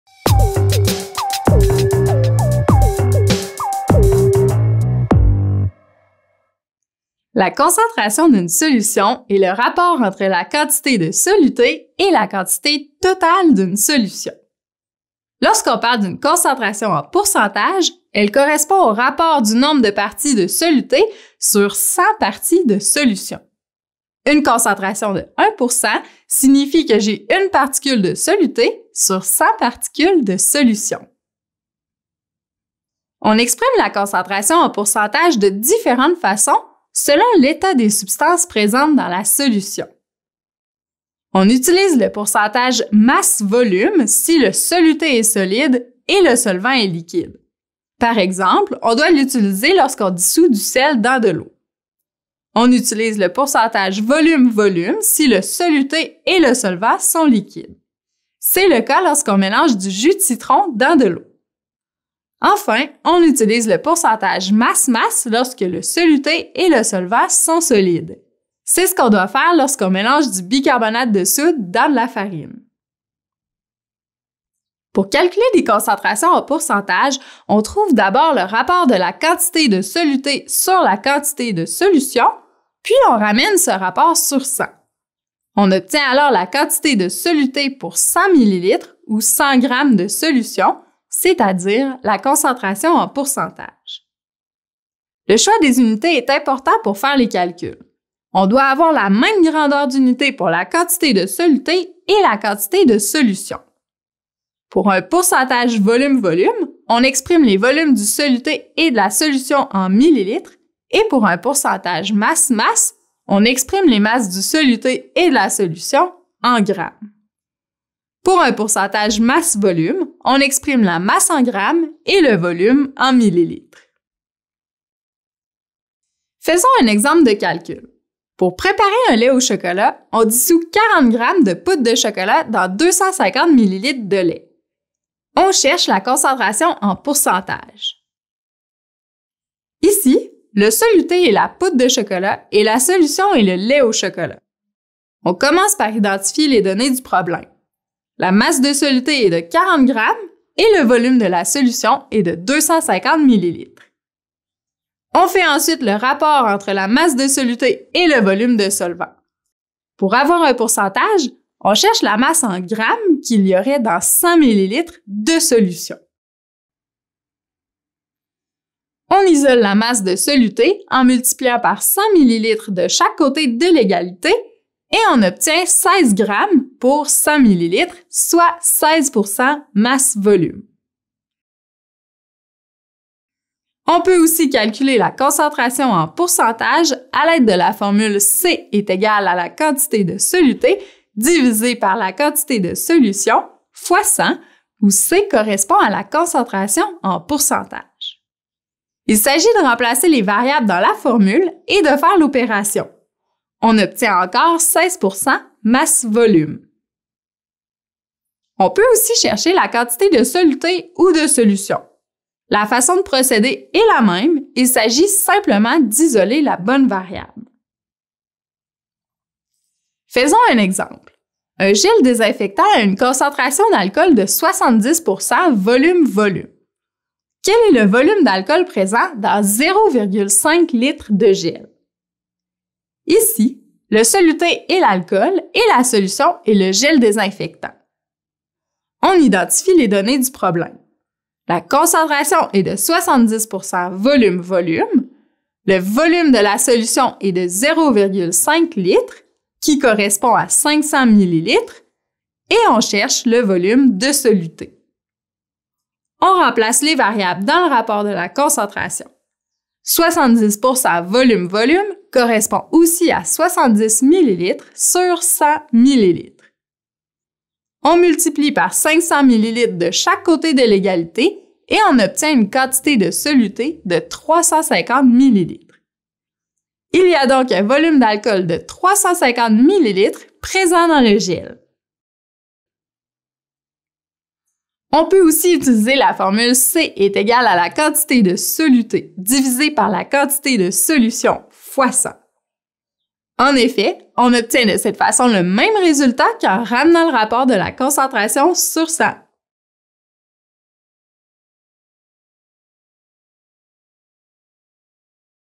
La concentration en pourcentage | Sciences et technologies | Alloprof
Comment calculer une concentration en pourcentage?
Toutes les explications sont dans cette vidéo!

Valérie t’explique toutes les notions pour comprendre les pourcentages masse/masse, masse/volume et volume/volume.

Découvre toutes les vidéos de sciences et technologies de…

Chapitres :
0:00 La concentration en pourcentage
0:32 Les unités de la concentration en pourcentage
1:14 Le calcul de la concentration avec un rapport
1:35 Les unités du soluté et du solvant
2:05 Exemple de calcul de concentration
3:03 Le calcul de la concentration avec la formule
3:27 Exemple de calcul de volume de soluté

Correction:
2:20 Techniquement, le lait au chocolat n’est pas une solution. En effet, ce n’est pas un mélange homogène, mais plutôt un colloïde. Ceci dit, le concept de concentration se comprend mieux avec un bon lait chocolaté!
2:39 Ici, on devrait entendre « volume de solution » et non « volume de solvant ».